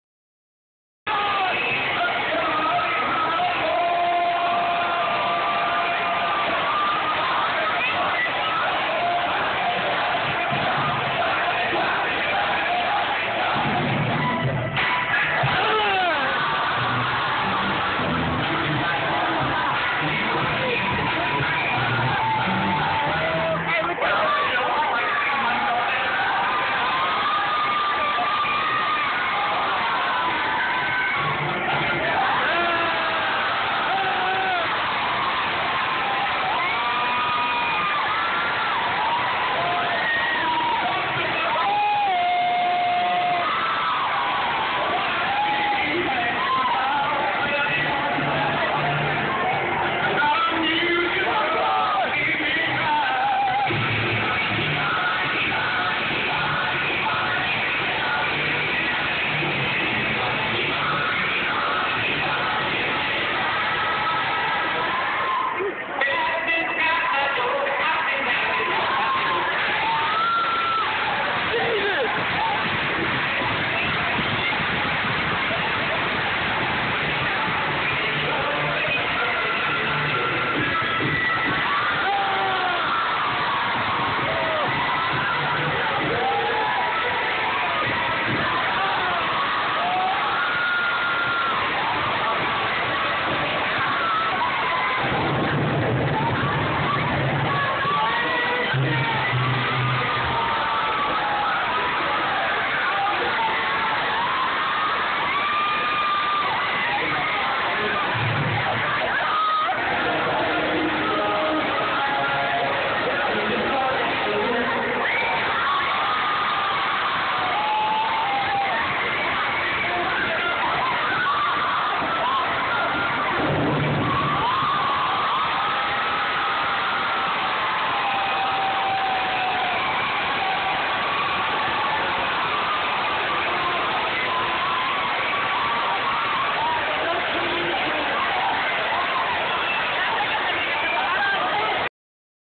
Homecoming Rally '12 Boys Soccer
Sonoma Varsity soccer boys dance at homecoming rally '11-'12. The do "moving it like Bernie", "the Charleston dance", "cat daddy", and "Celtic dancing"